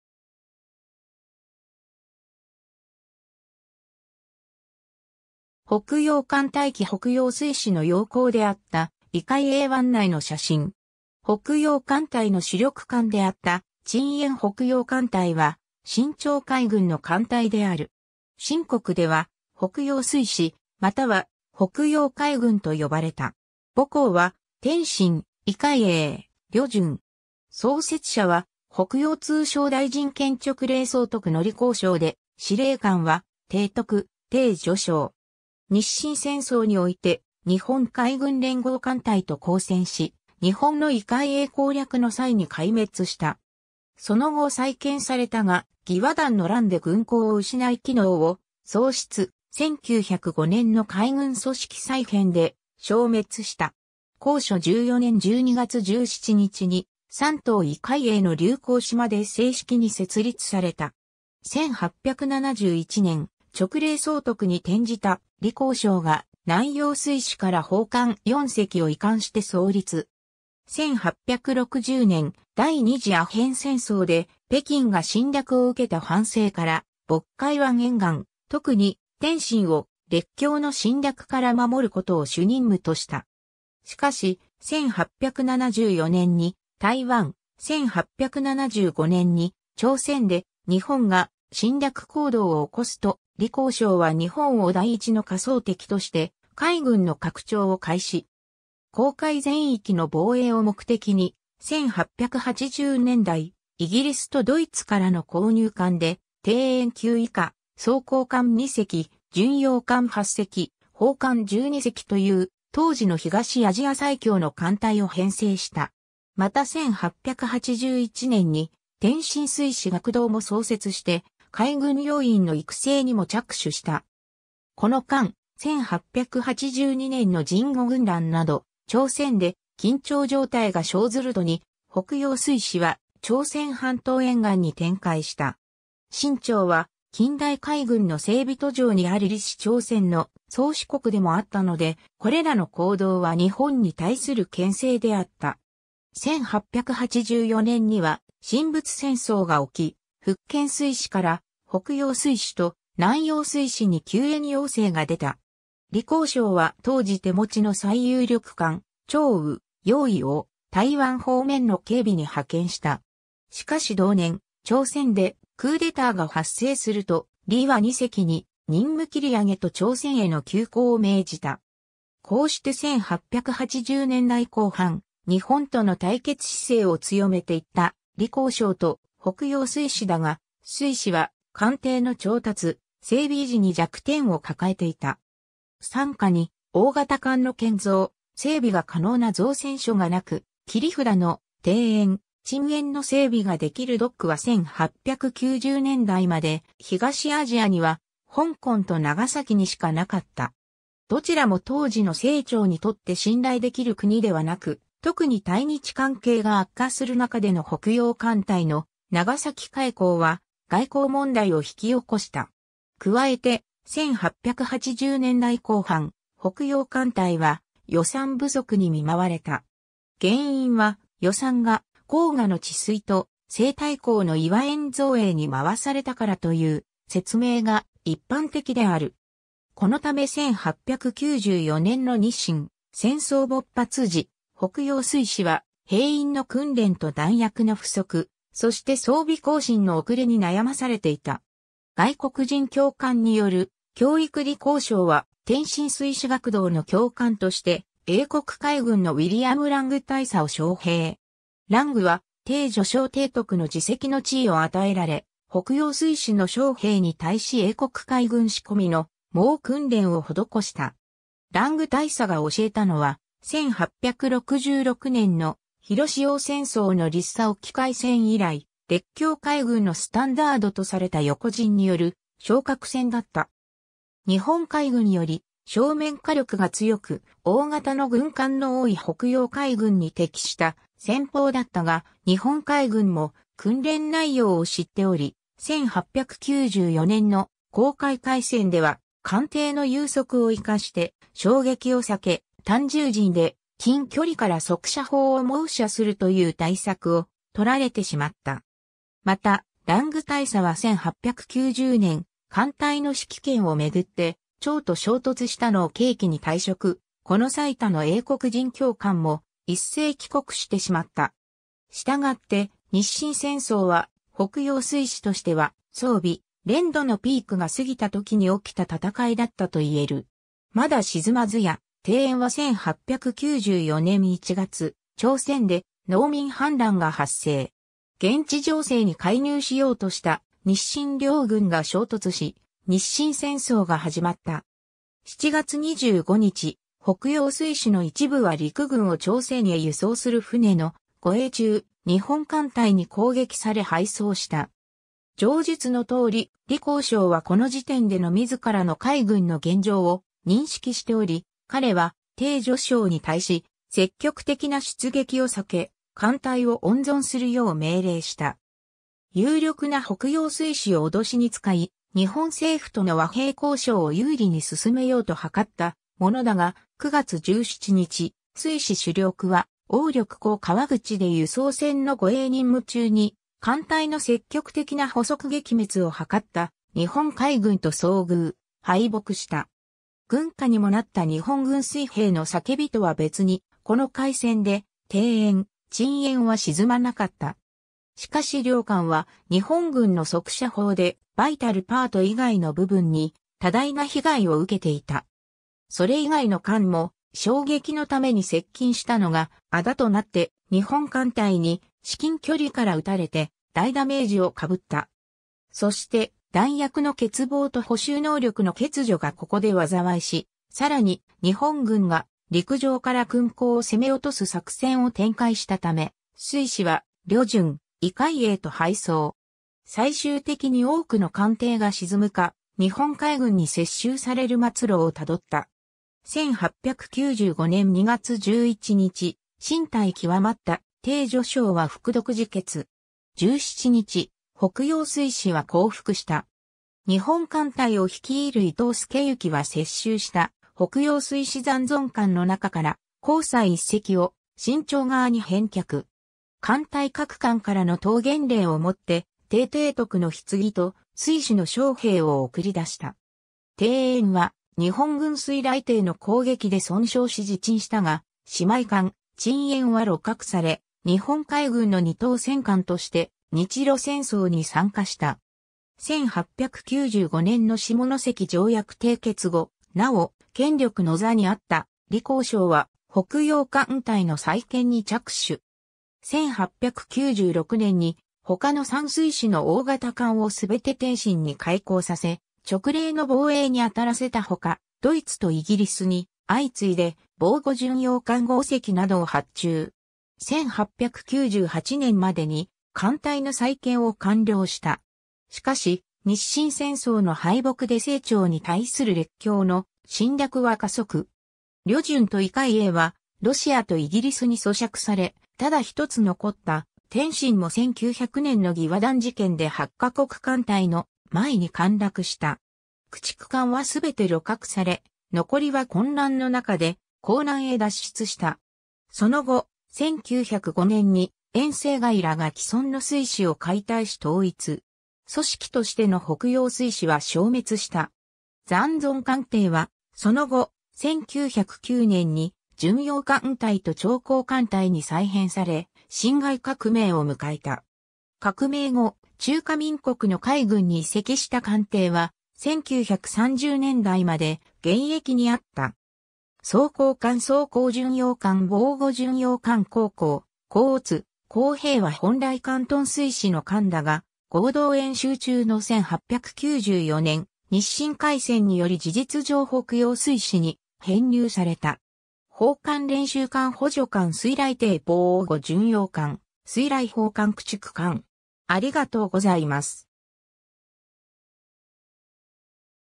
北洋艦隊
北洋艦隊旗（清国黄龍旗） 北洋水師の要港であった威海衛湾内の写真。
 北洋艦隊の主力艦であった「鎮遠」 北洋艦隊（ほくようかんたい）は、清朝海軍の艦隊である。
清国では北洋水師、または北洋海軍と呼ばれた。
母港は天津、威海衛、旅順。
創設者は北洋通商大臣兼直隷総督の李鴻章で司令官は提督丁汝昌。
日清戦争において日本海軍連合艦隊と交戦（黄海海戦、威海衛海戦）し、日本の威海衛攻略の際に壊滅した。
その後再建されたが、義和団の乱で軍港を失い機能を喪失、1905年の海軍組織再編で消滅した。
 光緒14年（1888年）12月17日に山東威海衛の劉公島で正式に設立された。
 1871年、直隷総督に転じた李鴻章（それまで彼は両江総督曽国藩の部下だった）が南洋水師から砲艦4隻を移管して創立。
1860年、第二次阿片戦争（アロー戦争）で北京が侵略を受けた反省から、渤海湾沿岸、特に天津を列強の侵略から守ることを主任務とした。
 しかし、1874年に台湾（台湾出兵）、1875年に朝鮮で日本が侵略行動（江華島事件）を起こすと、李鴻章は日本を第一の仮想敵として海軍の拡張を開始。
黄海全域の防衛を目的に、1880年代、イギリスとドイツからの購入艦で「定遠」級以下、装甲艦2隻、巡洋艦8隻、砲艦12隻という、当時の東アジア最強の艦隊を編成した。
また1881年に天津水師学堂も創設して海軍要員の育成にも着手した。
 この間、1882年の壬午軍乱など朝鮮で緊張状態が生ずる度に、北洋水師は朝鮮半島沿岸に展開した。
清朝は近代海軍の整備途上にある李氏朝鮮の宗主国でもあったので、これらの行動は日本に対する牽制であった。
1884年には清仏戦争が起き、福建水師から北洋水師と南洋水師に救援要請が出た。
李鴻章は当時手持ちの最有力艦、「超有」「揚威」（日清戦争時の主力、「定遠」級以下は未完成、または回航中に抑留されていた）を台湾方面の警備に派遣した。
しかし同年、朝鮮でクーデターが発生（甲申事変）すると、李は2隻に任務切り上げと朝鮮への急行を命じた。
 こうして1880年代後半、日本との対決姿勢を強めていった李鴻章と北洋水師だが、水師は艦艇の調達、整備維持に弱点を抱えていた。
傘下に大型艦の建造・整備が可能な造船所がなく、切り札の「定遠」「鎮遠」の整備ができるドックは1890年代まで東アジアには香港と長崎にしかなかった（旅順のドックは日清戦争まで完工しなかった）。
どちらも当時の清朝にとって信頼できる国ではなく（軍艦の調達過程で、李鴻章はドイツ寄りになっていた）、特に対日関係が悪化する中での北洋艦隊の長崎回航は、外交問題を引き起こした（長崎事件）。
 加えて、1880年代後半、北洋艦隊は予算不足に見舞われた。
原因は予算が黄河の治水と、西太后の頤和園造営に回されたからという説明が一般的である。
 このため1894年の日清戦争勃発時、北洋水師は兵員の訓練と弾薬の不足、そして装備更新の遅れに悩まされていた。
 外国人教官による教育 李鴻章は、天津水師学堂の教官として英国海軍のウィリアム・ラング大佐を招聘。
ラングは丁汝昌提督の次席の地位を与えられ、北洋水師の将兵に対し英国海軍仕込みの猛訓練を施した。
ラング大佐が教えたのは、1866年の普墺戦争のリッサ沖海戦以来、列強海軍のスタンダードとされた横陣による衝角戦だった。
（仮想敵の）日本海軍より正面火力が強く、大型の軍艦の多い北洋海軍に適した戦法だったが、日本海軍も訓練内容を知っており、1894年の黄海海戦では艦艇の優速を活かして衝撃を避け、単縦陣で近距離から（1880年代後半に性能が向上した）速射砲を猛射するという対策を採られてしまった。
 またラング大佐は1890年、艦隊の指揮権を巡って丁と衝突したのを契機に退職、この際他の英国人教官も一斉帰国してしまった（ドイツ、アメリカなど他国籍の教官は残った）。
従って日清戦争は、北洋水師としては装備・練度のピークが過ぎた時に起きた戦いだったと言える。
 まだ沈まずや、定遠は 1894年1月、朝鮮で農民反乱が発生（甲午農民戦争）。
現地情勢に介入しようとした日清両軍が衝突し、日清戦争が始まった。
7月25日、北洋水師の一部は陸軍を朝鮮へ輸送する船の護衛中、日本艦隊に攻撃され敗走した（豊島沖海戦）。
 上述の通り、李鴻章はこの時点での自らの海軍の現状を認識しており、彼は丁汝昌に対し積極的な出撃を避け、艦隊を温存するよう命令した。
有力な北洋水師を脅しに使...